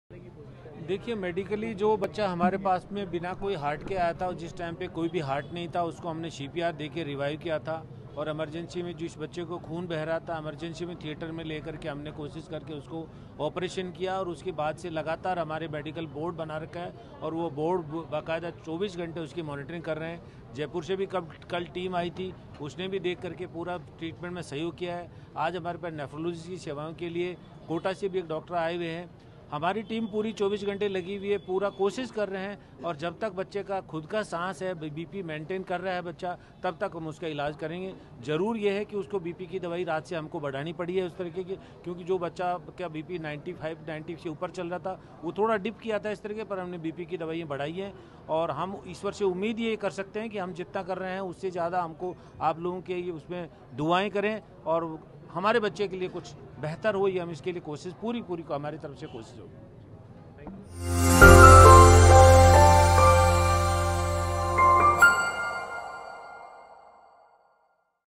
Udaipur Violence Update: उदयपुर कांड में डॉ आर एल सुमन का बयान आया सामने, कहा-जब तक सांस है...
About this Video:

Udaipur Violence Update: उदयपुर कांड में डॉ आर एल सुमन का बयान आया सामने, कहा-जब तक सांस है... | 18 August 2024